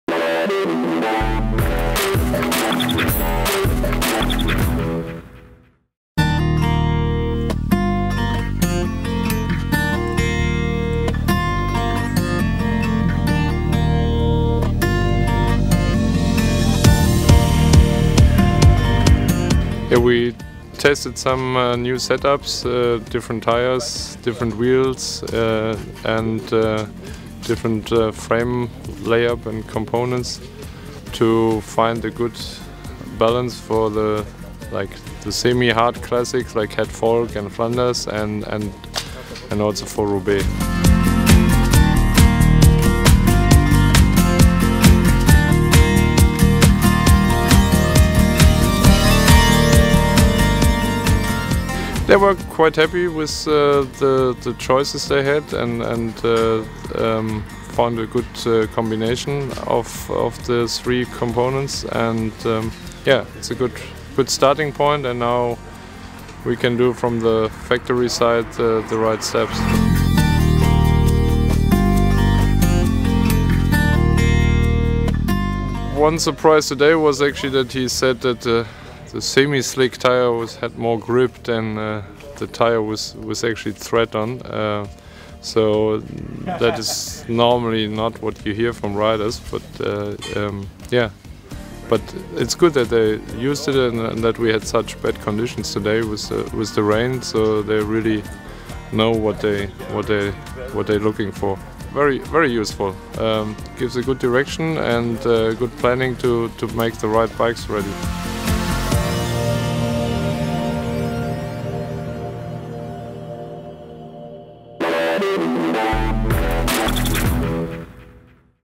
Orica-GreenEDGE testing for spring classics
In the winter of 2012 Orica-GreenEDGE was already testing different set-ups for the spring classics of 2013 like Tour of Flanders and Paris Roubaix. Technical director and former pro Lars Teutenberg explains how they test different set-ups like wheels, tires and bike fitting in the rain.